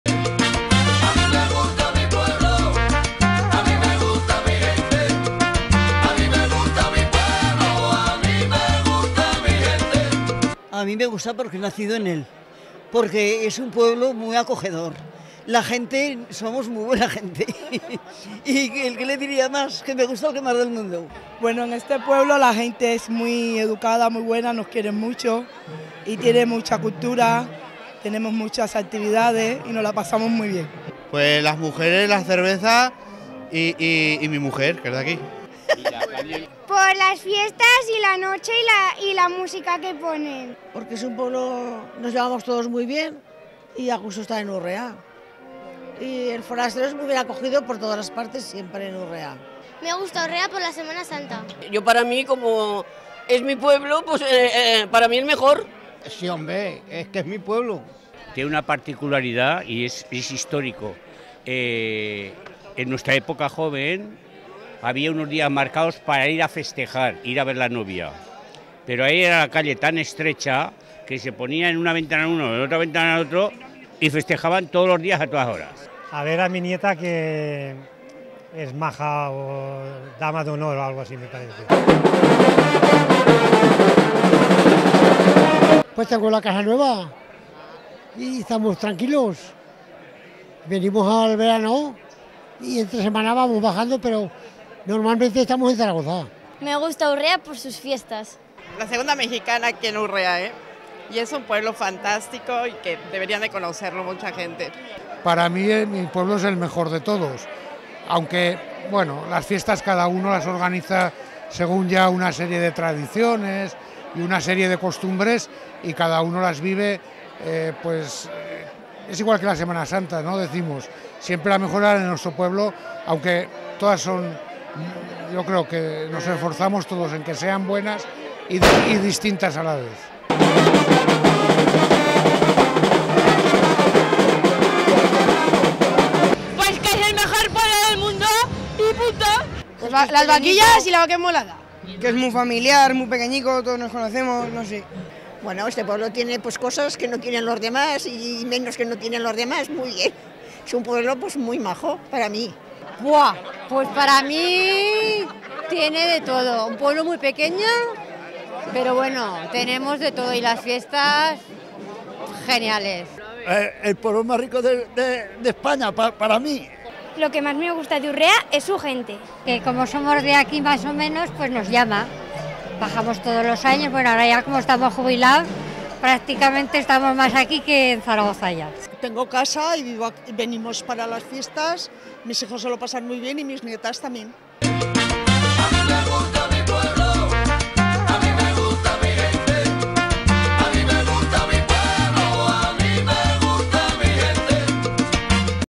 La Comarca.tv - Me gusta mi pueblo Urrea de Gaén
En fiestas los pueblos se multiplican en población, es el caso de Urrea de Gaén en la comarca del Bajo Martín y allí sus vecinos nos cuentan con animación que su pueblo es el mejor.